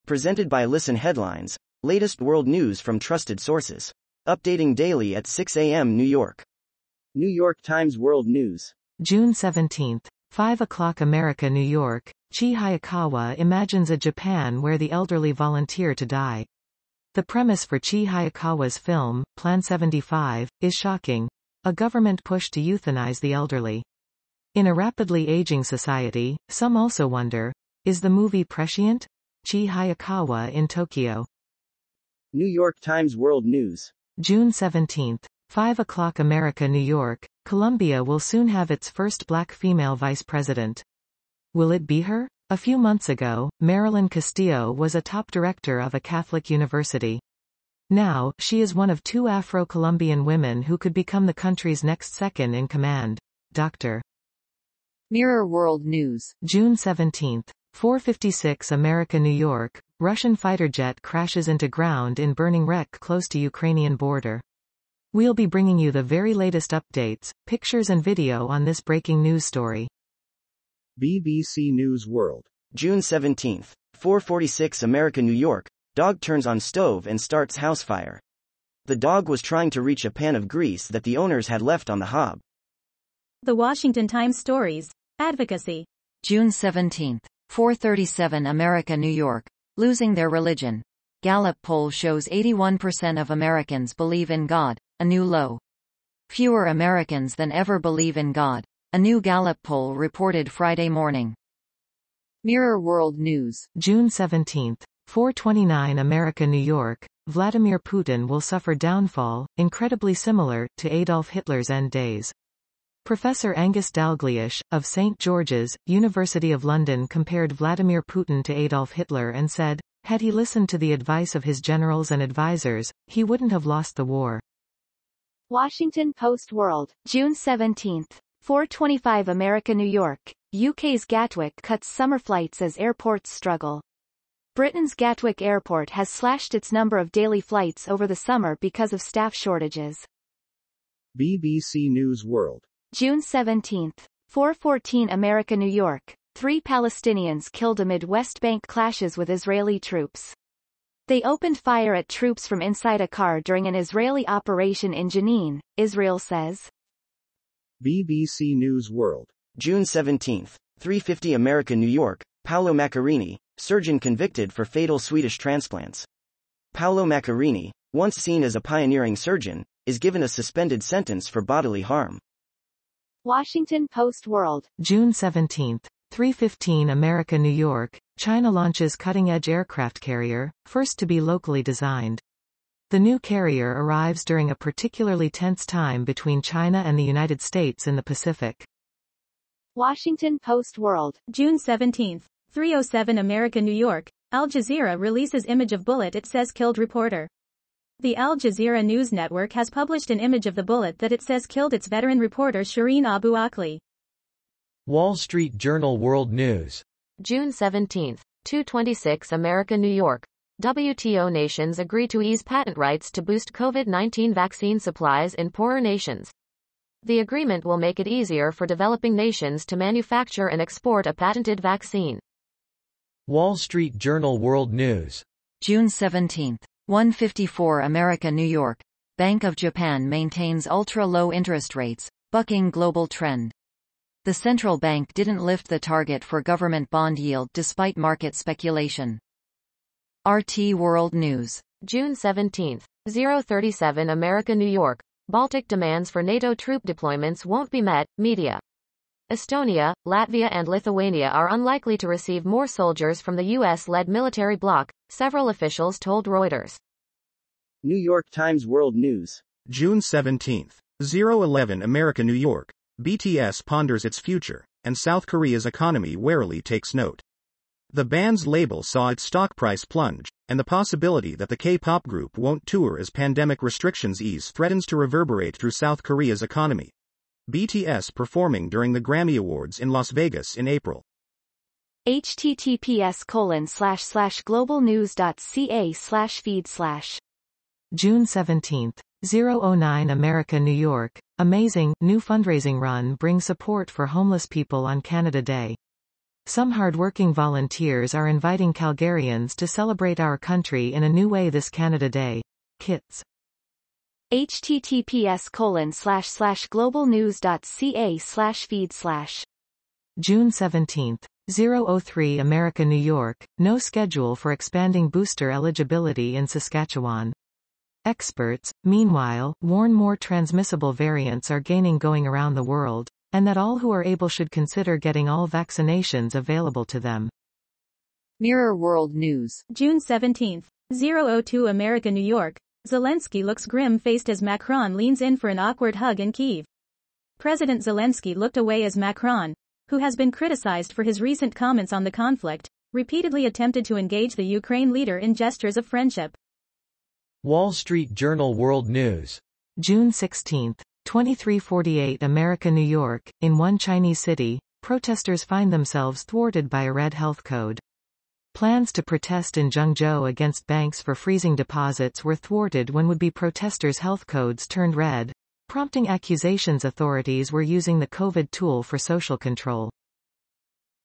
World News 2022-06-17-0530 (made with Spreaker)
presented by listen headlines, latest world news from trusted sources.

Length: 06:11

 item link #1

  Updating daily at 6 AM New York

 item link #2

  Chie Hayakawa Imagines a Japan Where the Elderly Volunteer to Die

Motoko Rich

New York Times World News @ June 17. 05:00 - America New York
 item link #3

  Colombia Will Soon Have Its First Black Female Vice President. Will It Be Her?

Genevieve Glatsky

 item link #4

  Russian fighter jet crashes into ground in burning wreck close to Ukrainian border

Mirror World news @ June 17. 04:56 - America New York
 item link #5

  Dog turns on stove and starts house fire

B B C News World @ June 17. 04:46 - America New York
 item link #6

  Losing their religion: Gallup poll shows 81% of Americans believe in God, a new low

Mark A. Kellner

The Washington Times stories: Advocacy @ June 17. 04:37 - America New York
 item link #7

  Vladimir Putin will suffer downfall 'incredibly similar' to Adolf Hitler's end days

Mirror World news @ June 17. 04:29 - America New York
 item link #8

  UK's Gatwick cuts summer flights as airports struggle

Associated Press

Washington Post World @ June 17. 04:25 - America New York
 item link #9

  Three Palestinians killed amid West Bank clashes with Israeli troops

B B C News World @ June 17. 04:14 - America New York
 item link #10

  Paolo Macchiarini: Surgeon convicted for fatal Swedish transplants

B B C News World @ June 17. 03:50 - America New York
 item link #11

  China launches cutting-edge aircraft carrier, first to be locally designed

Lily Kuo

Washington Post World @ June 17. 03:15 - America New York
 item link #12

  Al Jazeera releases image of bullet it says killed reporter

Associated Press

Washington Post World @ June 17. 03:07 - America New York
 item link #13

  WTO Nations Agree to Ease Patent Rights to Boost Covid-19 Vaccine Supplies in Poorer Nations

Wall Street Journal world news @ June 17. 02:26 - America New York
 item link #14

  Bank of Japan Maintains Ultra-Low Interest Rates, Bucking Global Trend